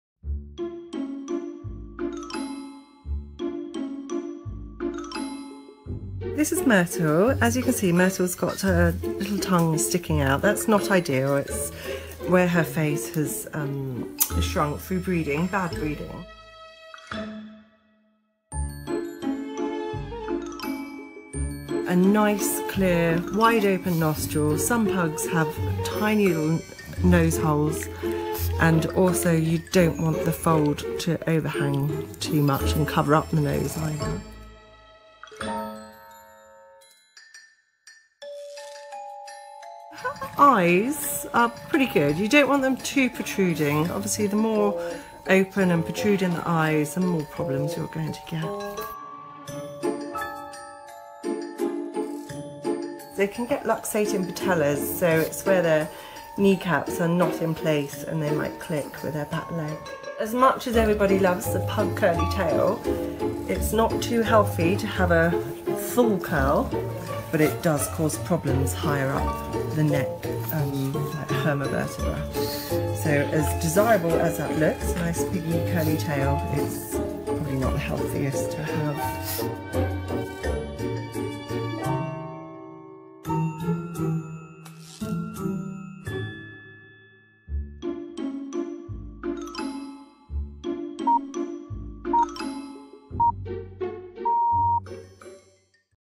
Why pugs' tongues shouldn't actually stick out
It might be cute but pugs whose tongues stick out point to a birth defect